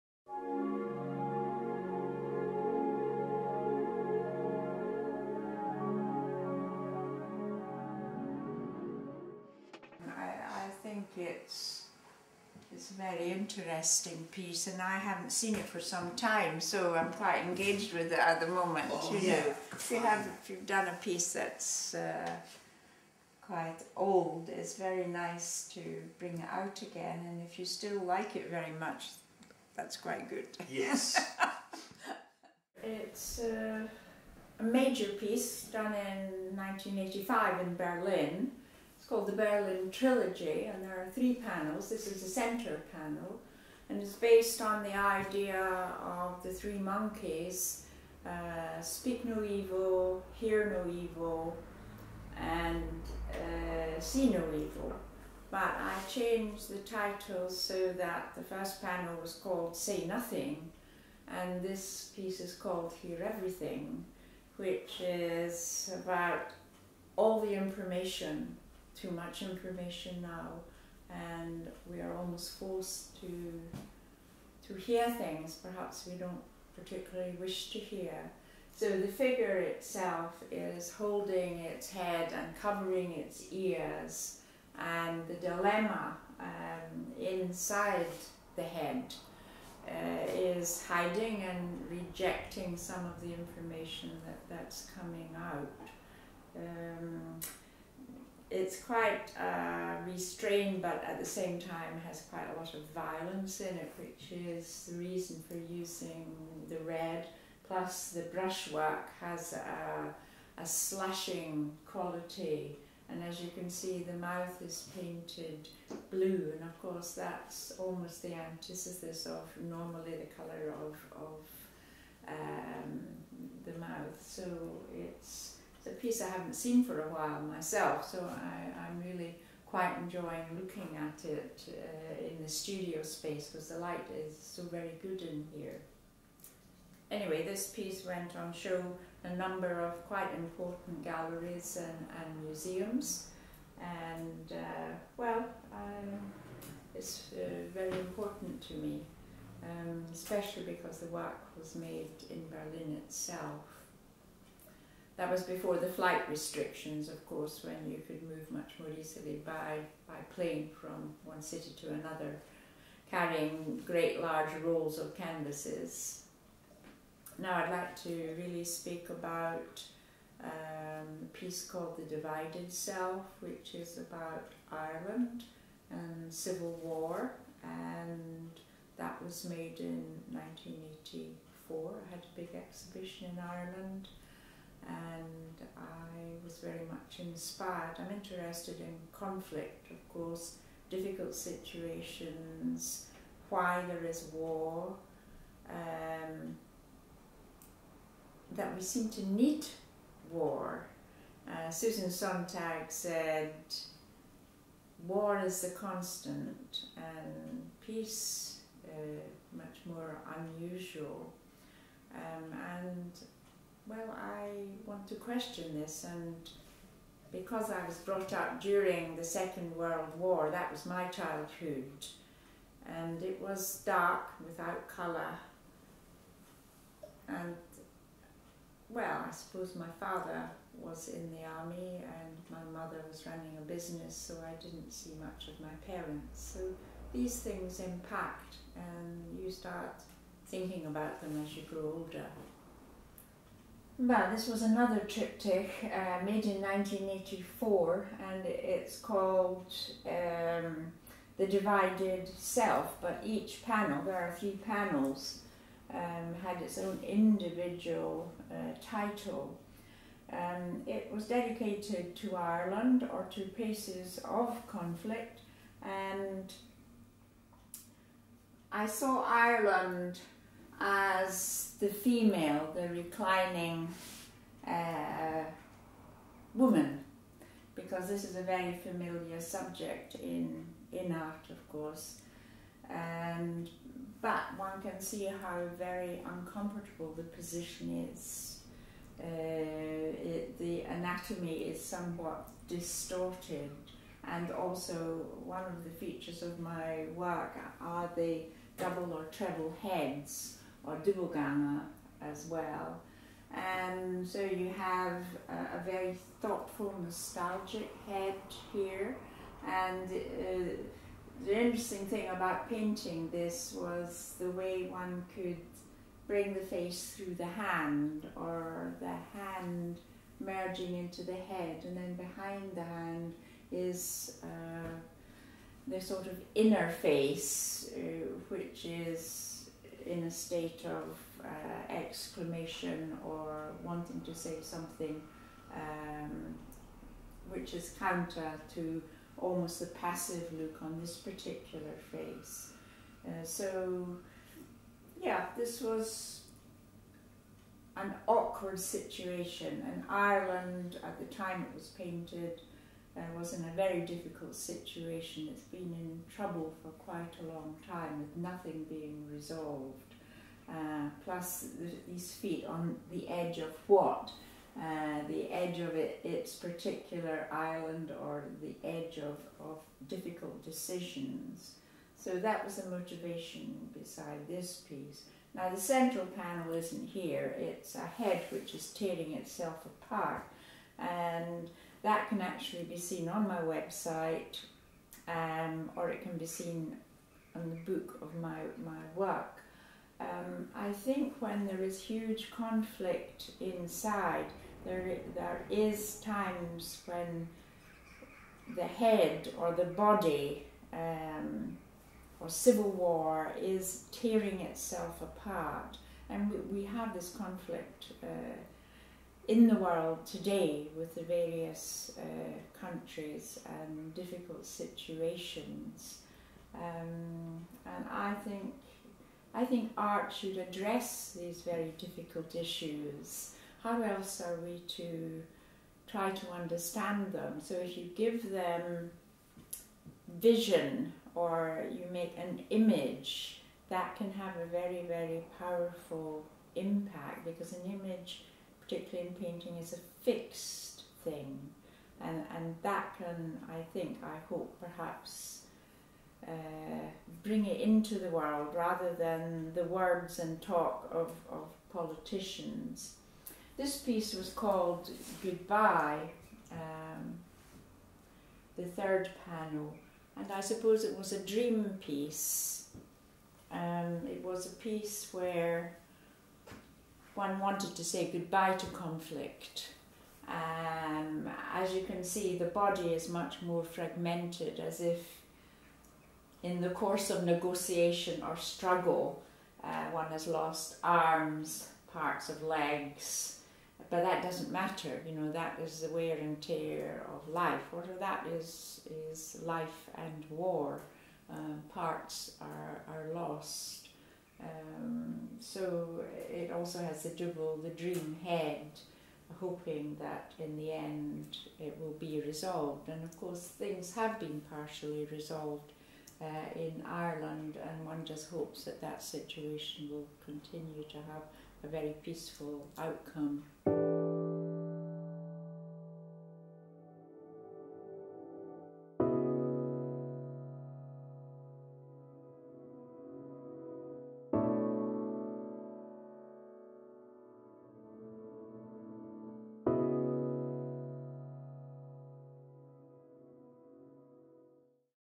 Lys Hansen - Missing Pieces
Perthshire artist Lys Hansen talks about her earlier works, triptychs Berlin Trilogy and The Divided Self (1985). Filmed at her Braco studio on 9 November 2012.